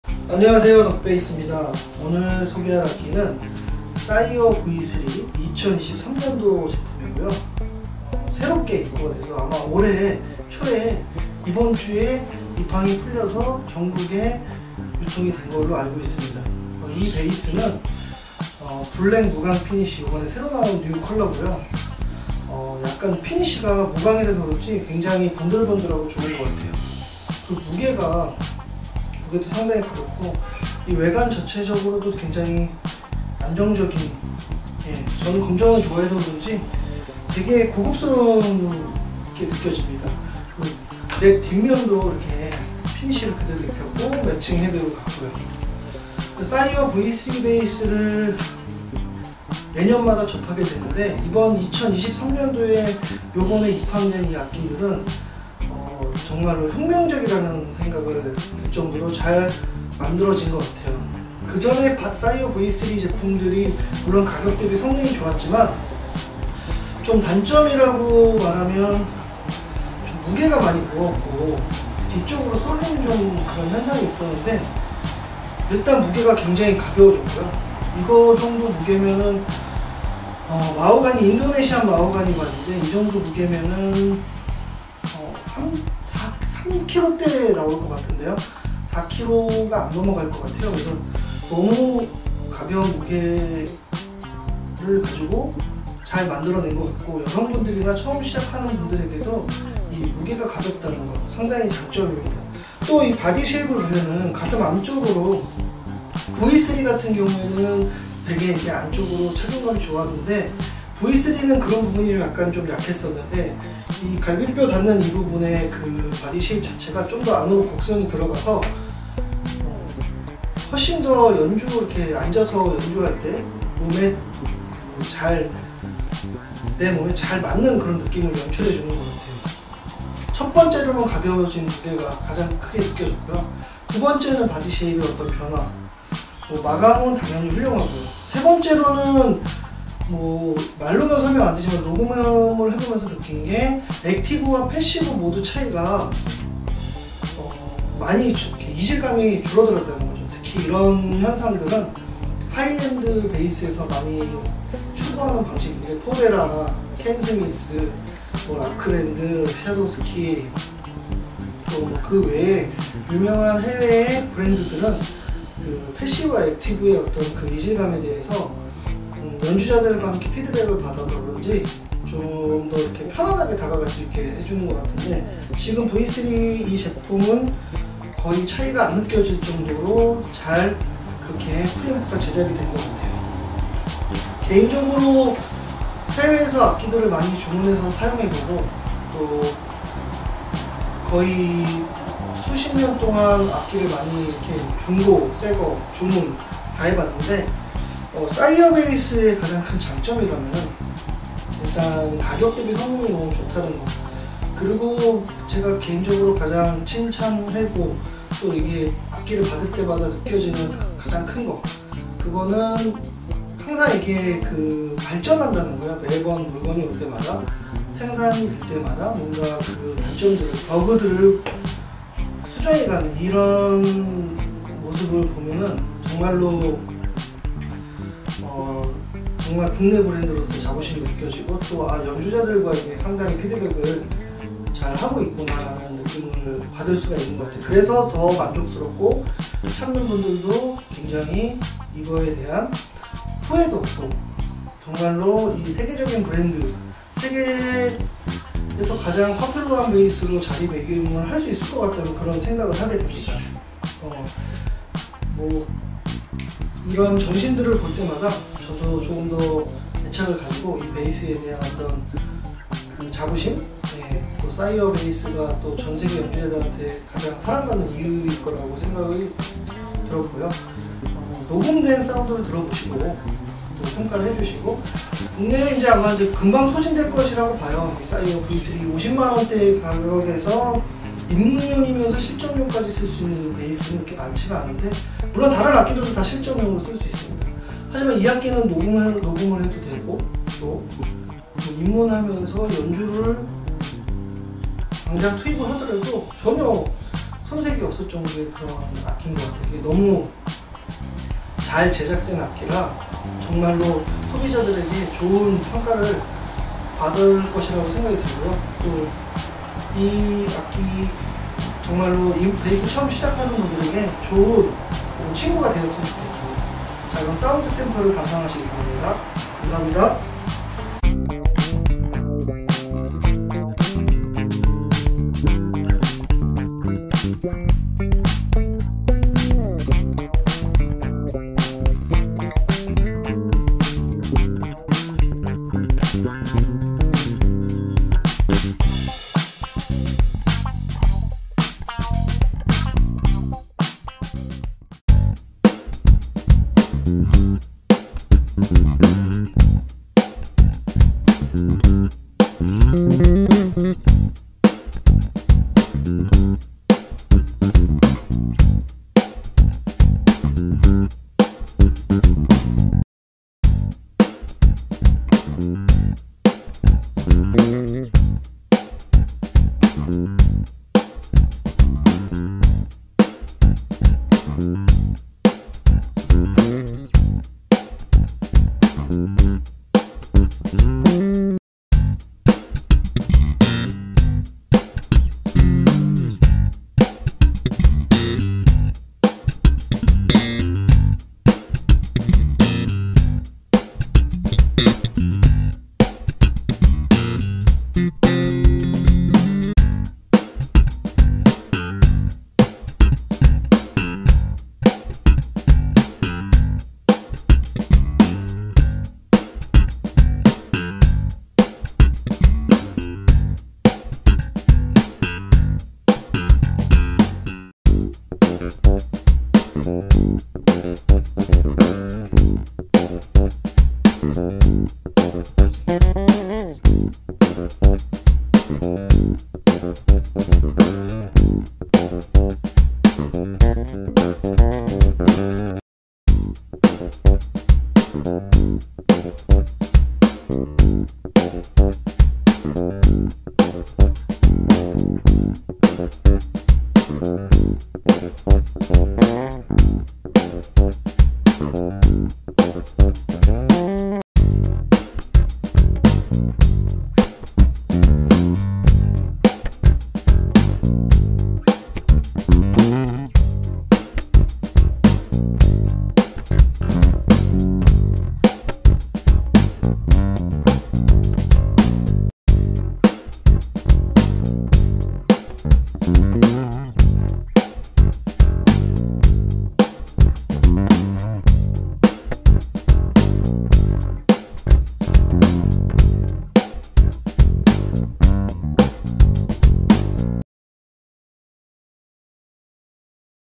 50만원 재즈베이스 혁명 "2023 Sire V3 4string"
안녕하세요. 덕베이스입니다. 2023년도에 새롭게 입고된 사이어 v3 무광 블랙컬러가 입고되었습니다. 일단 왹관적으로도 정말 훌륭하다는 말을 하고 싶습니다. 그리고 무엇보다 V3 무거운 무게가 정말 가볍게 변화를 이루었다는 것이고, 가장 좋은 점은 액티브 모드와 패시브 모드 전환시 왜곡없이 자연스럽게 들린다는 것입니다.

사이어 베이스 진화에 박수를 보내고 응원을 보내고 싶습니다.